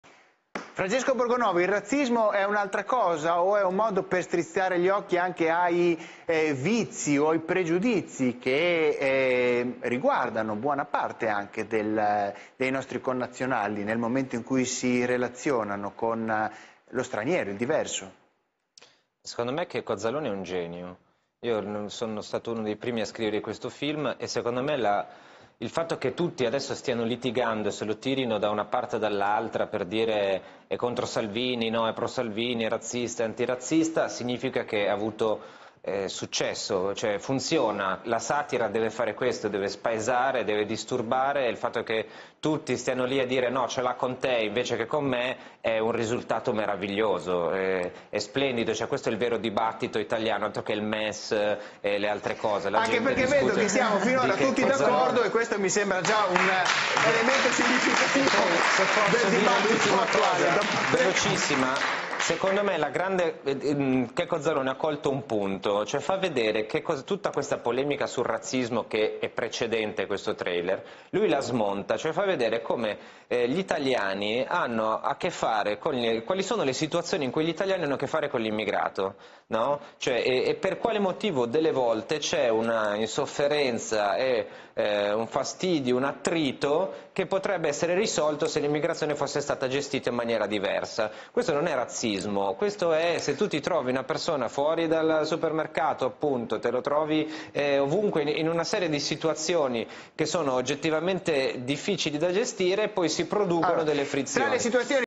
Francesco Borgonovo: "Checco Zalone è un genio, la satira deve spaesare, ci fa vedere che ...
Francesco Borgonovo (La Verità): "Adesso se lo stanno litigando, è razzista, è antirazzista, funziona. Un risultato meraviglioso, questo è il vero dibattito italiano, altro che il MES".